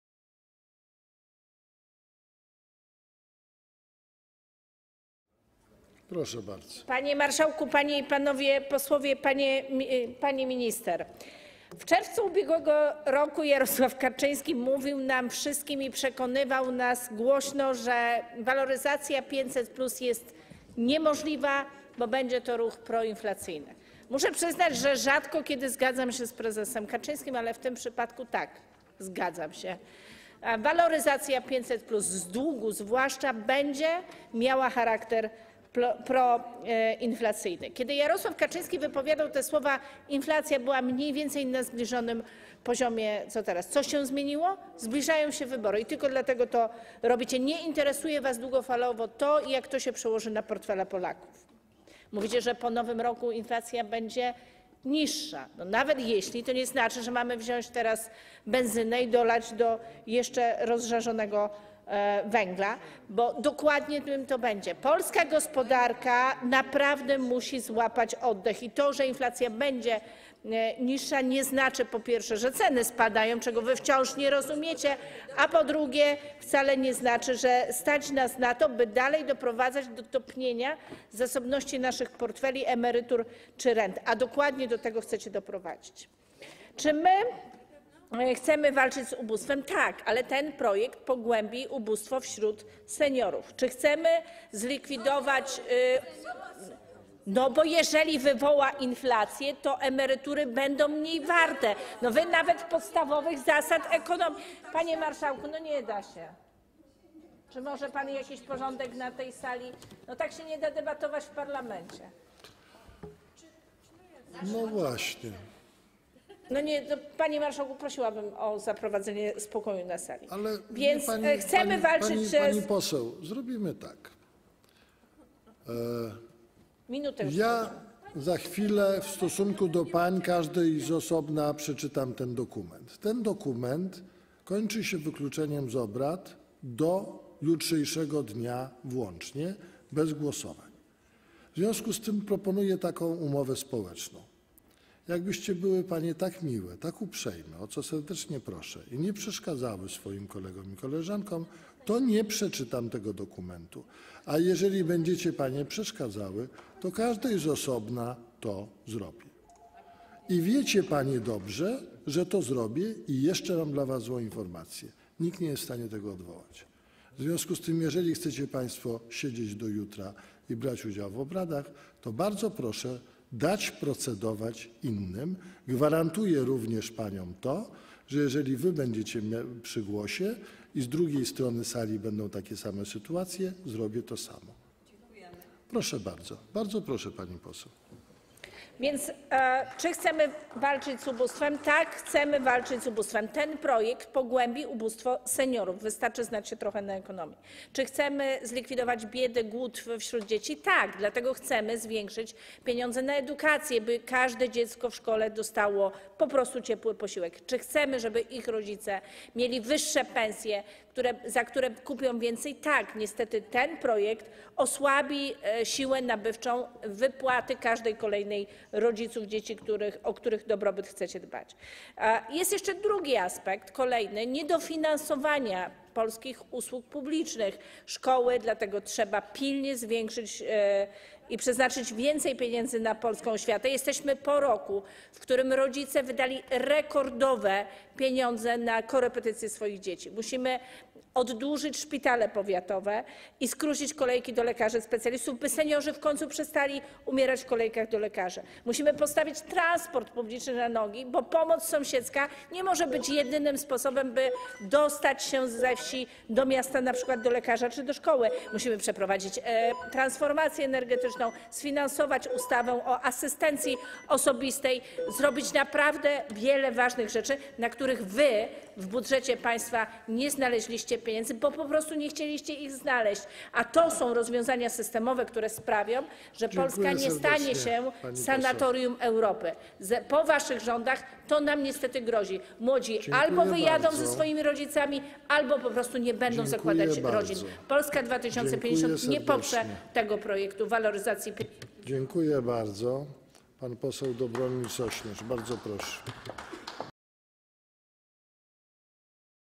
Paulina Hennig-Kloska - świadczenie 800+
Paulina Hennig-Kloska - świadczenie 800+ [6 lipca 2023 r.]

P. 12. - Sprawozdanie Komisji o rządowym projekcie ustawy o zmianie ustawy o pomocy państwa w wychowywaniu dzieci